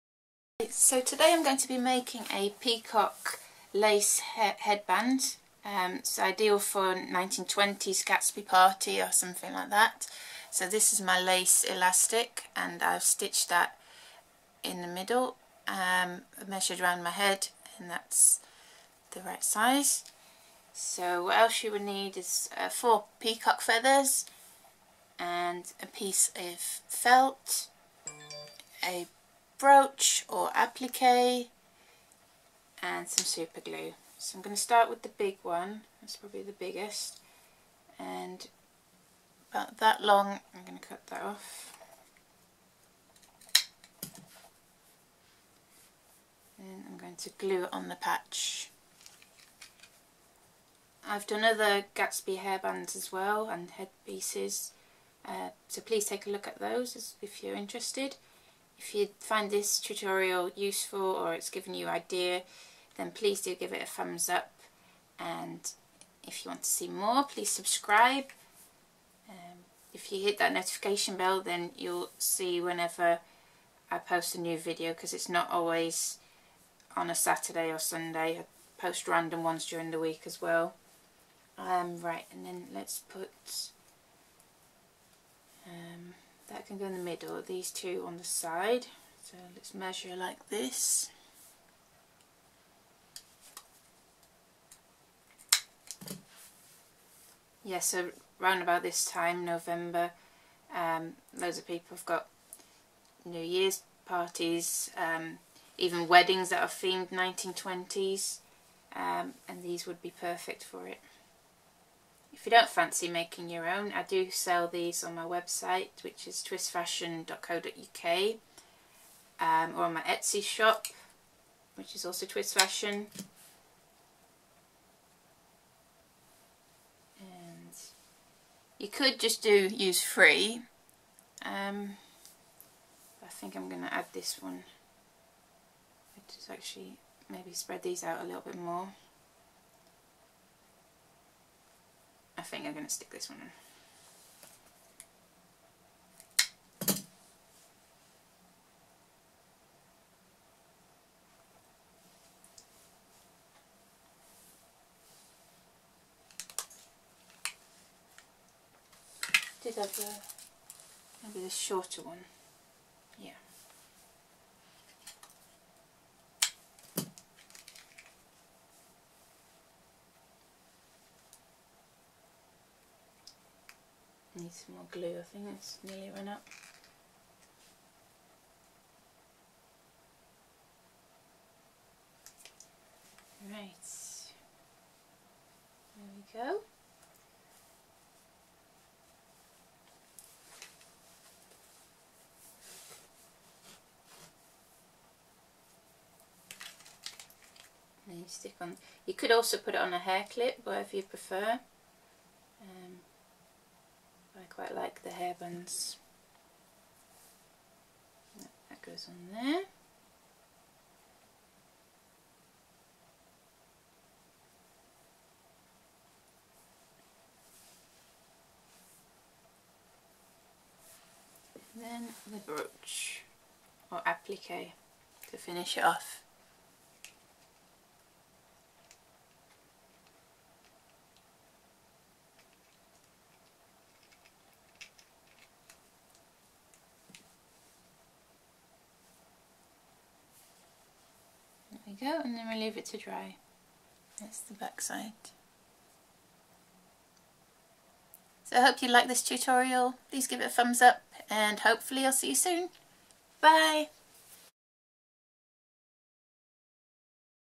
How to make a 1920's Peacock Feather headband tutorial - Easy and Fast DIY - Millinery Headpiece
This DIY video, shows you exactly how to make a beautiful
 easy and quick 1920s gatsby Peacock feather headband/hairband.

All you need is:
4x Peacock feathers
Diamante applique
Elastic Headband
Superglue
Felt patch

We also sell lots of other gatsby headbands and fascinator hats, please take a look on the website

or in my Etsy shop